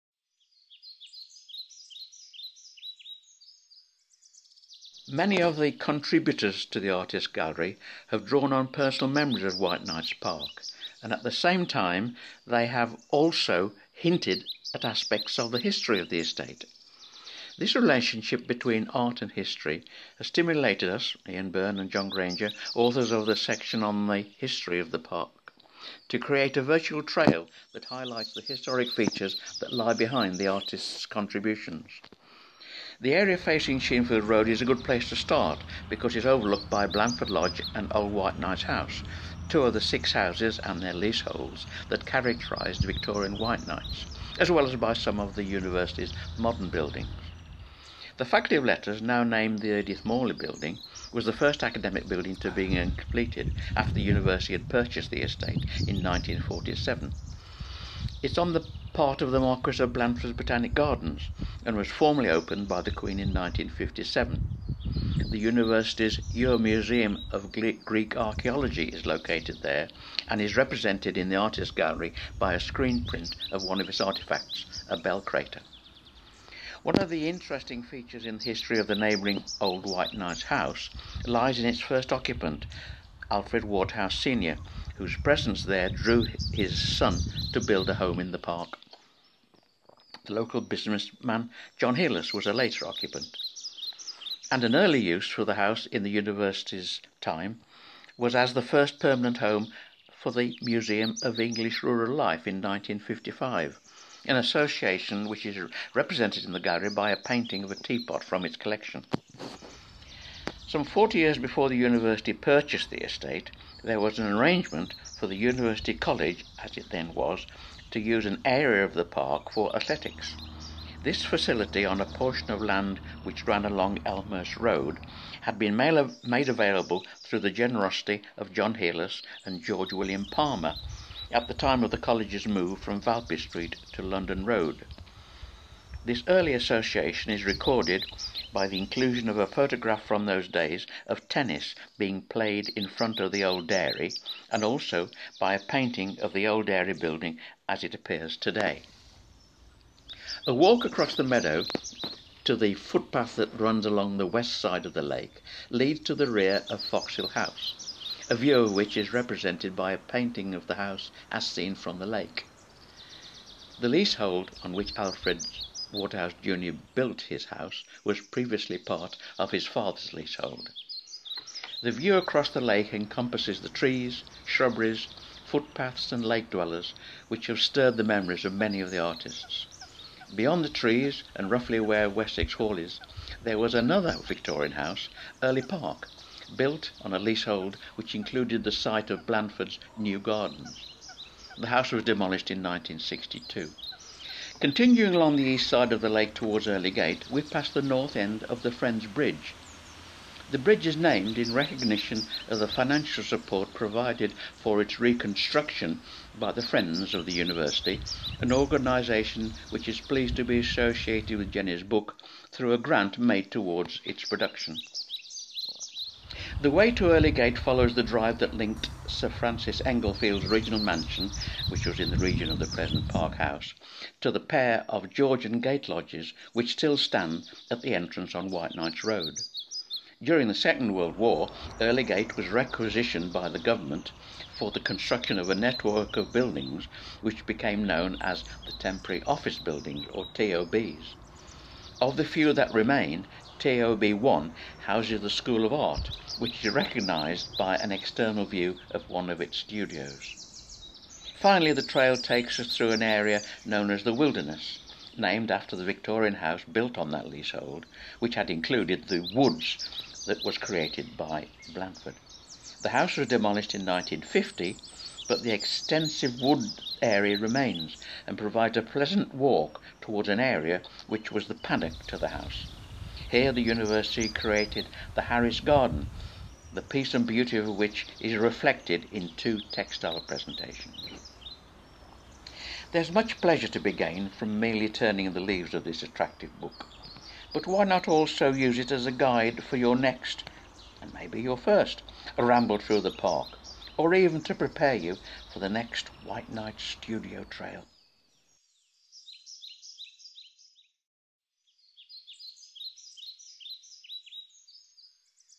A tour around the Whiteknights campus
Filmed to mark the publication of the book 'The Art and History of Whiteknights' this video, produced by John Grainger and Ian Burn, takes you on a tour of the Whiteknights campus of the University of Reading, and tells the stories behind some of the key historical buildings and sites, many of which provided inspiration for the artworks featured in the book. The video tour points out buildings and places which feature in some of the artworks, including The Ure Museum, The site of the Old Dairy, Foxhill House, Whiteknights Lake, TOB1, The Harris Garden, and The Wilderness.